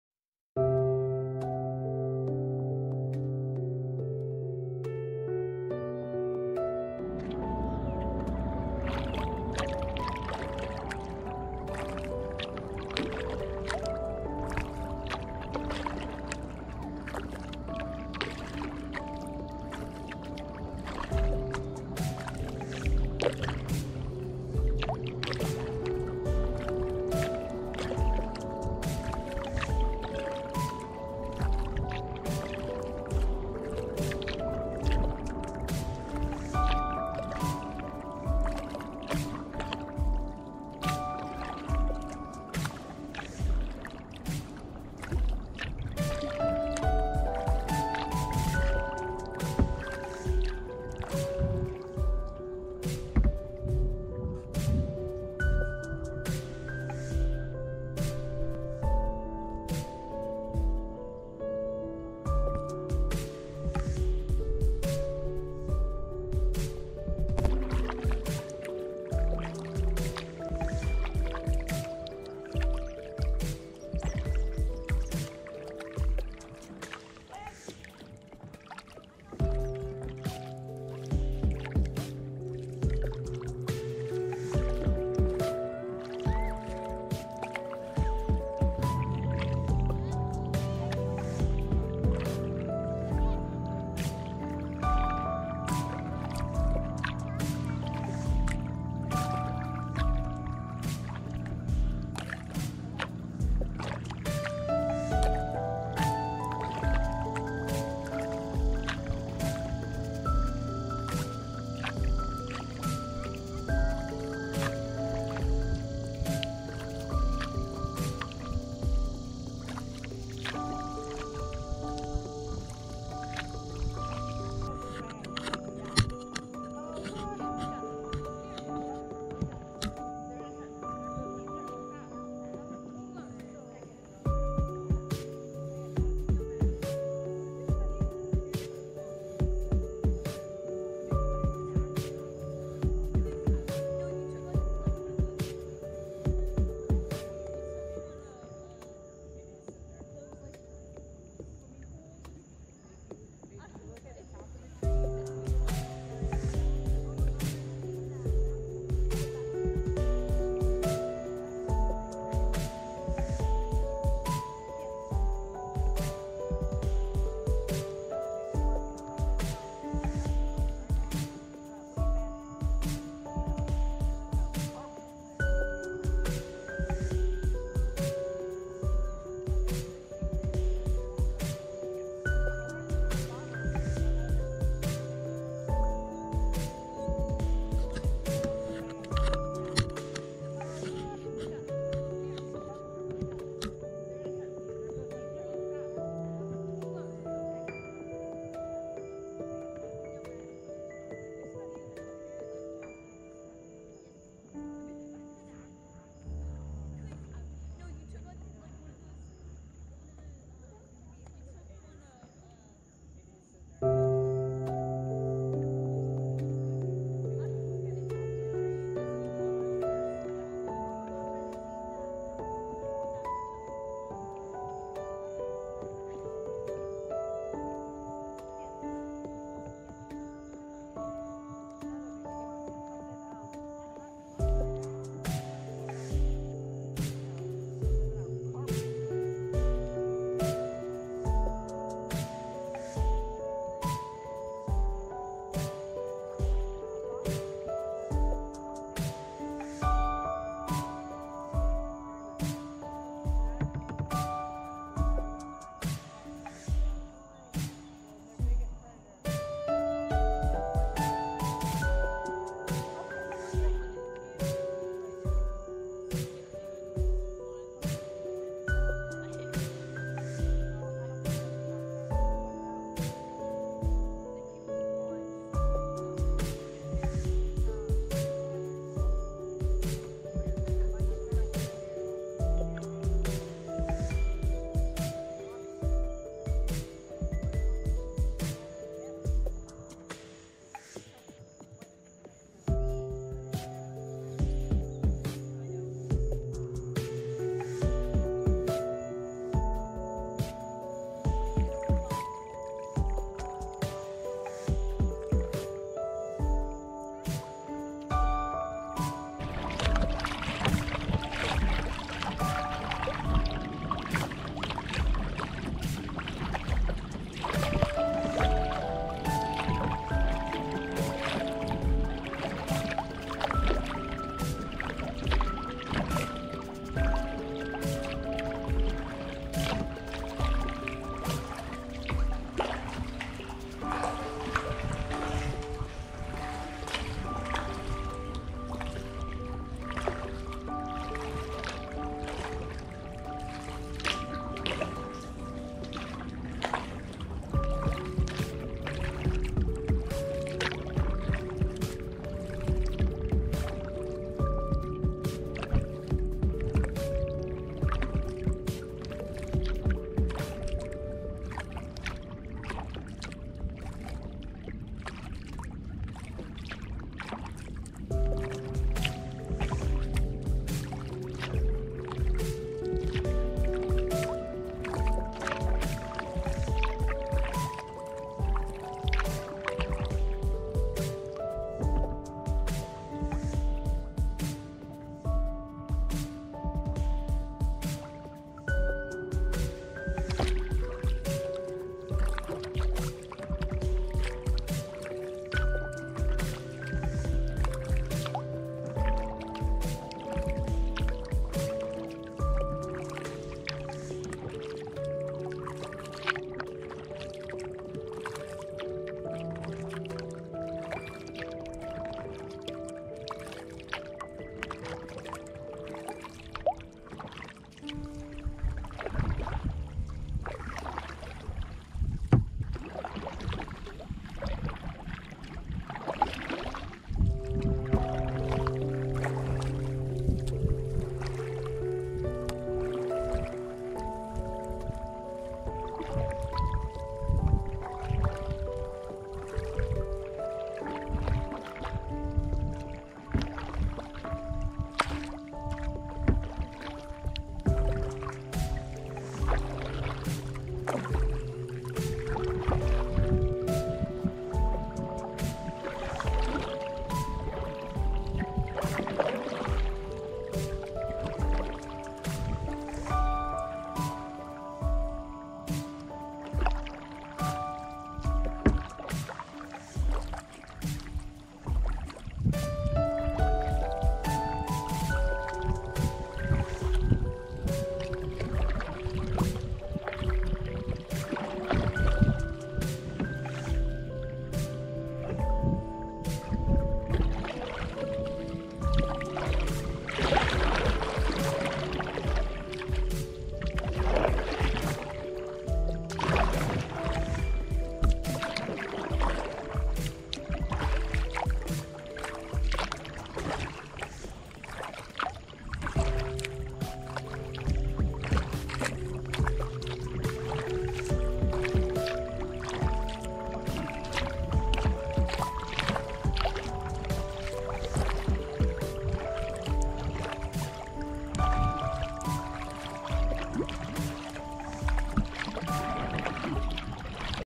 Paddling at Nickajack Cave
Paddling at the cave to watch the bats, and back across the lake to the campsite in the pitch black.